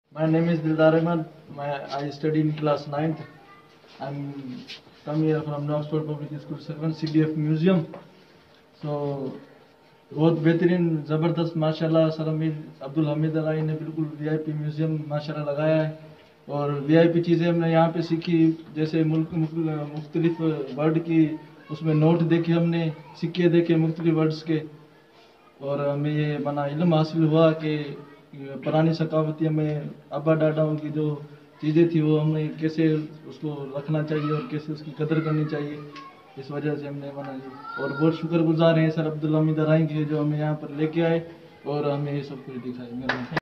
Dildar Ahmed Class Ninth
Dildar Ahmed views about CDF Museum Sakrand
views of Mr. Abdul Faheem about CDF Museum
Students of class ninth along with class teacher Mr. Abdul Faheem of CDF Schooling System campus near SDO wapda office Sakrand (New Oxford Public School Sakrand)visited the CDF Museum Sakrand on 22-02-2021. Abdul Hameed Arain Chairman CDF-Pakistan delivering the lecture and Briefing  to students about CDF Museum Sakrand.